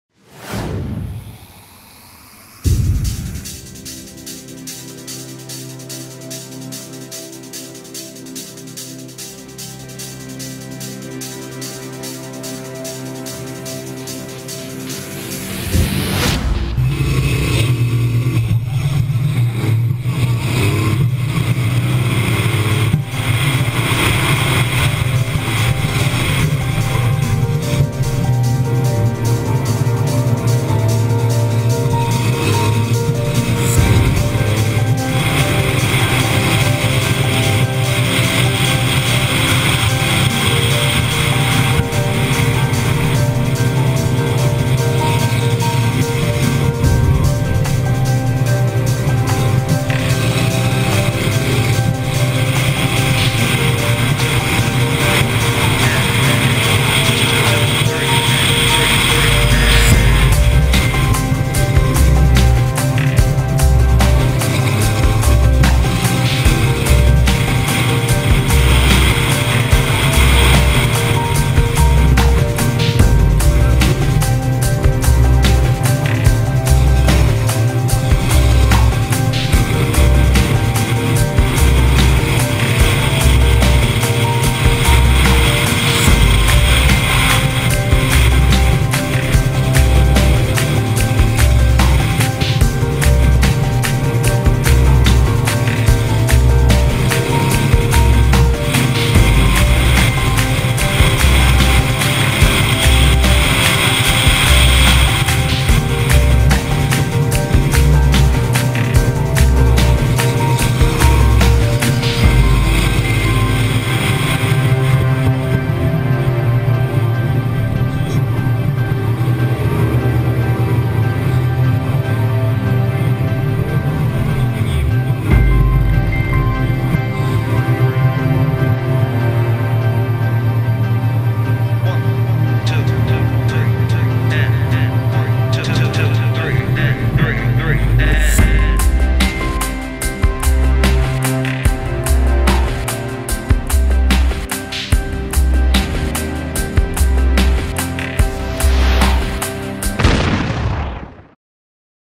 Hołowczyc vs Peterhansel (on-board camera) VERVA Street Racing 2011
Wyścig uliczny Hołowczyc vs Peterhansel okiem naszej kamery umieszczonej na samochodzie Hołka.